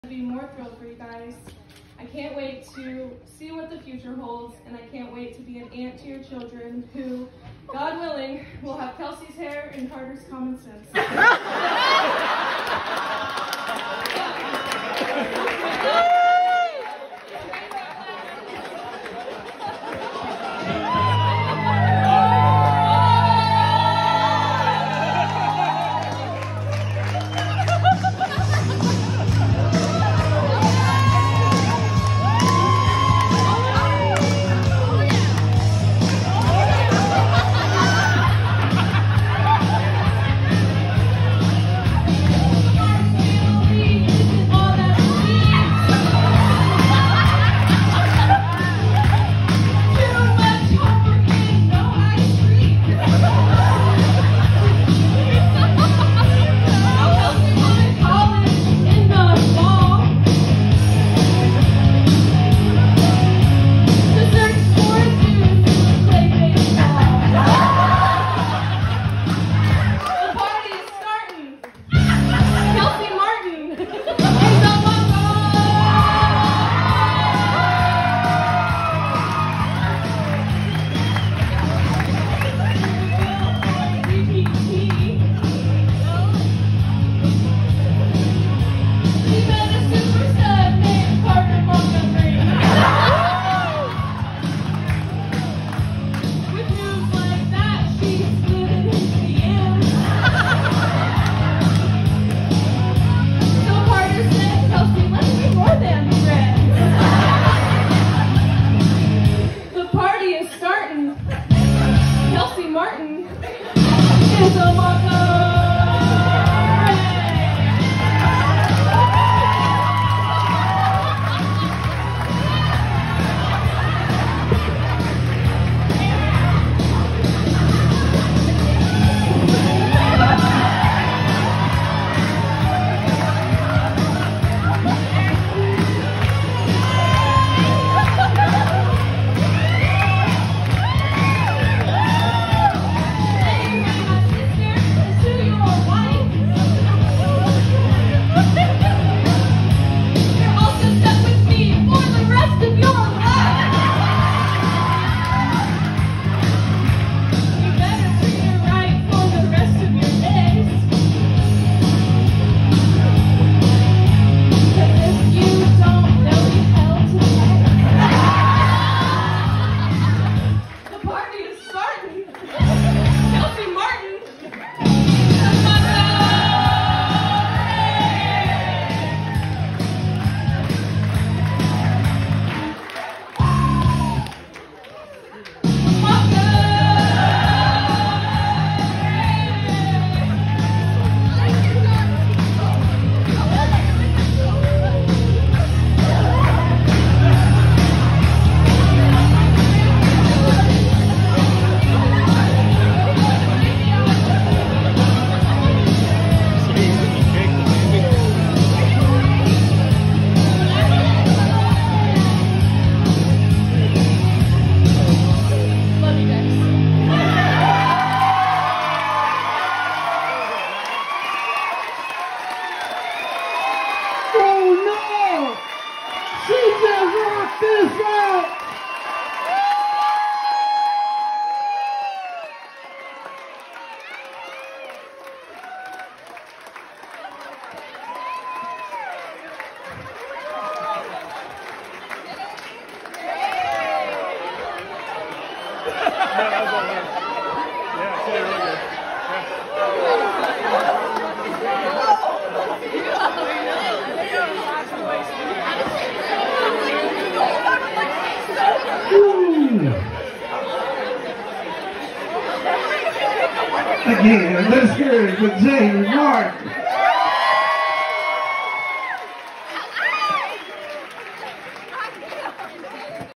Matron of Honor performance written by Jamie for her twin sister, Kelsey on May 15, 2021
Jamie wrote the words to this song well over a year ago & neither Kelsey nor Carter had any idea she was going to do anything more than make a speech. Talk about a surprise!  You can definitely see it on their faces!!
Jamie is 35 weeks pregnant with her second child during this performance. As Kelsey stated, “She did this sober!” Lol

The song:

The Martin family isn’t all that it seems
Too much homework and no ice cream

So Kelsey went to college in the fall
To search for a dude who played baseball

The party is startin’
Kelsey Martin
Is a Montgomery

They went to Maryville for DPT
She met a super stud named Carter Montgomery
With moves like that, she slid in his DMs
So Carter said, “Kelsey, let’s be more than friends”

Kelsey Martin
Is a Montgomery

Today you made my sister into your wife
You’re also stuck with me for the rest of your life

You better treat her right for the rest of your days
‘Cause if you don’t, there’ll be hell to pay

Kelsey Martin
Is a Montgomery

Montgomery